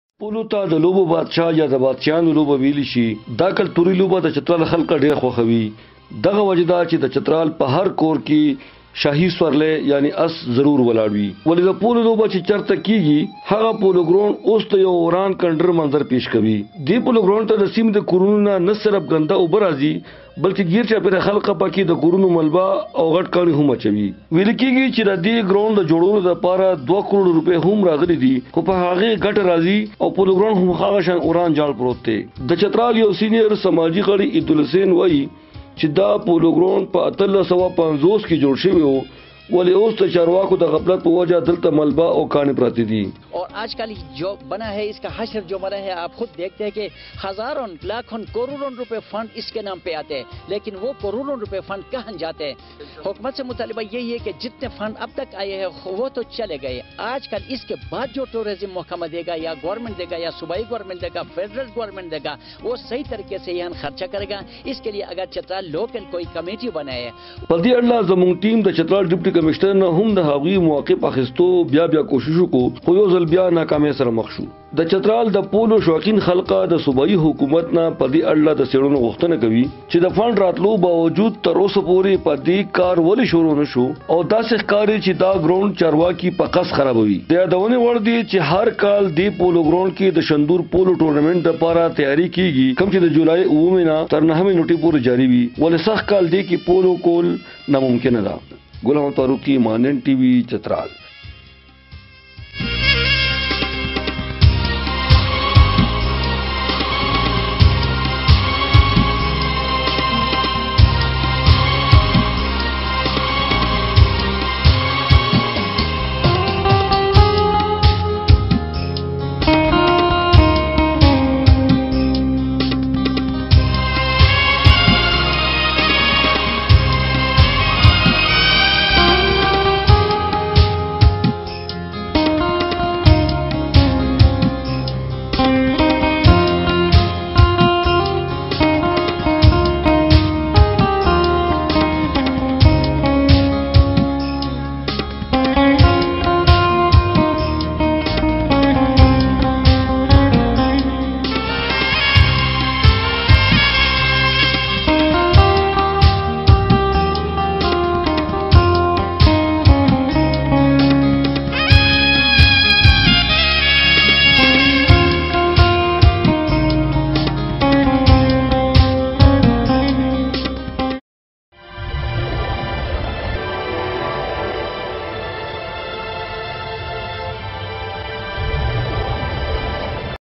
Polo Ground became a Desolate Ruins.
چترال میں پولو گراؤنڈ ویران کھنڈر بن گیا۔